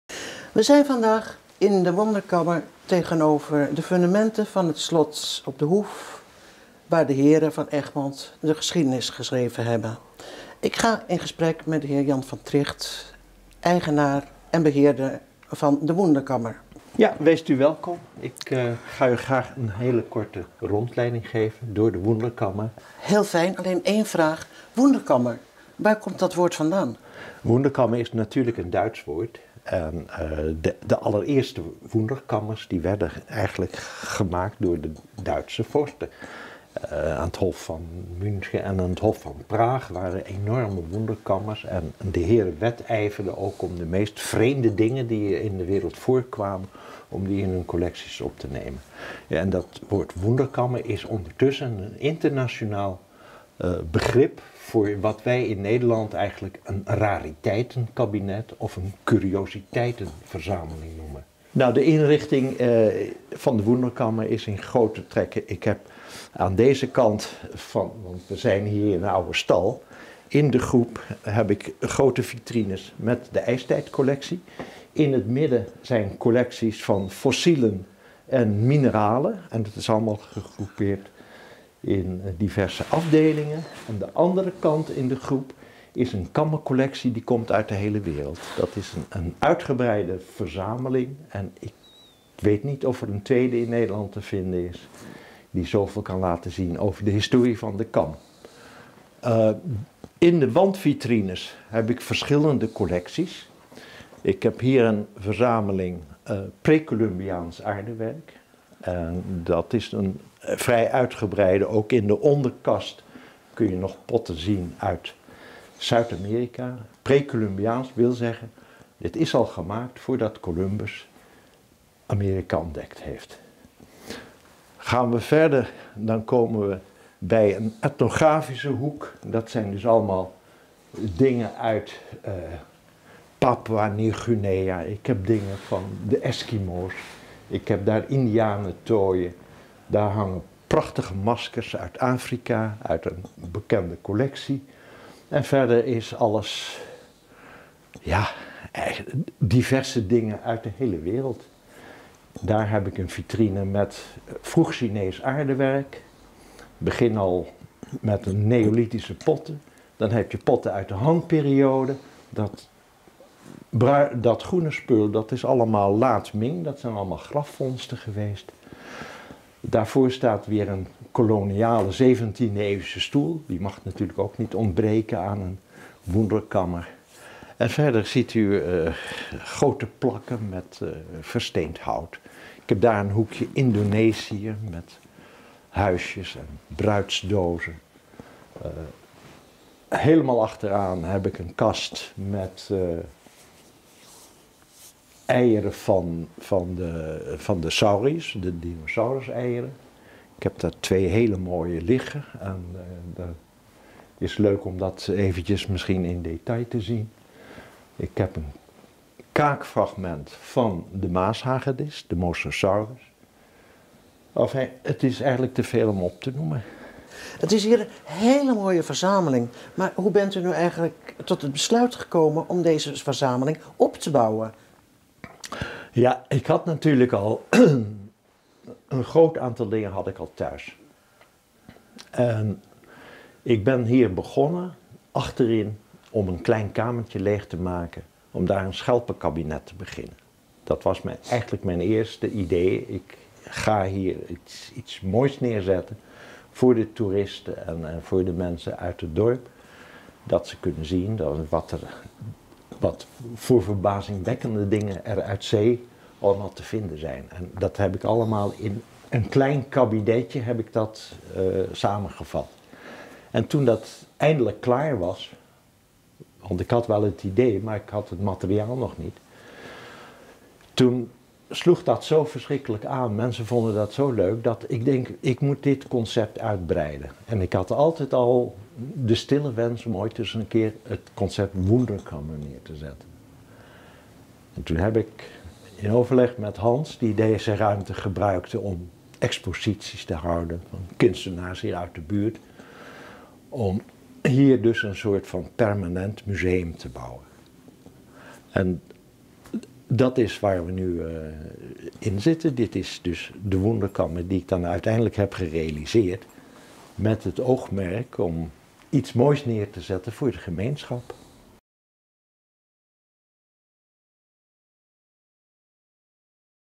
Jan van Trigt over het museum Wunderkammer in Egmond aan den Hoef
Jan van Trigt van Hoeve Overslot in Egmond aan den Hoef over zijn museum Wunderkammer Nicolaes Witsen. Hier zijn objecten uit een groot deel van de wereld te zien. Van fossielen en mineralen uit zijn ijstijdcollectie, Colombiaans en vroeg-Chinees aardewerk tot indianentooien, maskers uit Afrika, een koloniale 17e eeuwse stoel, objecten uit Indonesië en eieren van dinosauriërs.

Hoe lang het museum nog in Egmond is gevestigd was op 19 december 2021 nog onzeker. Naar verwachting gaat een nieuwe huurder gebruikmaken van deze ruimte.